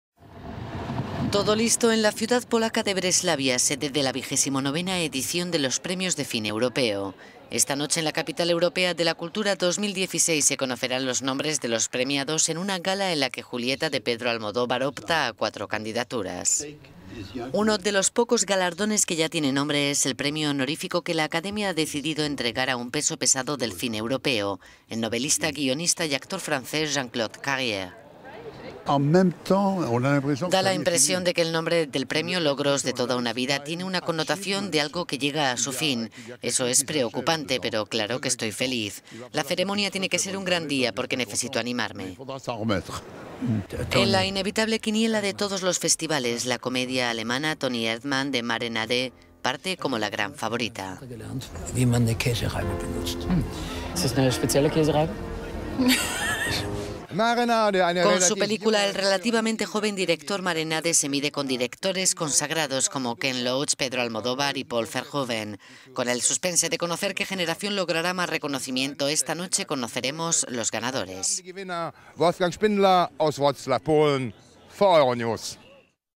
"Toni Erdmann" parte como favorita en los Premios de Cine Europeo
Todo está listo en la ciudad polaca de Breslavia, sede de la 29 edición de los Premios de Cine Europeo.

Esta noche en la capital europea de la cultura 2016 se conocerán los nombres de los premiados, en una gala en la que la que "Julieta", de Pedro Almodóvar, opta a cuatro candidaturas.

Uno de los pocos galardones que ya tiene nombre es el premio honorífico que la Academia ha decidido entregar a un peso pesado del cine europeo, el novelista, guionista y actor francés, Jean-Claude Carrière. …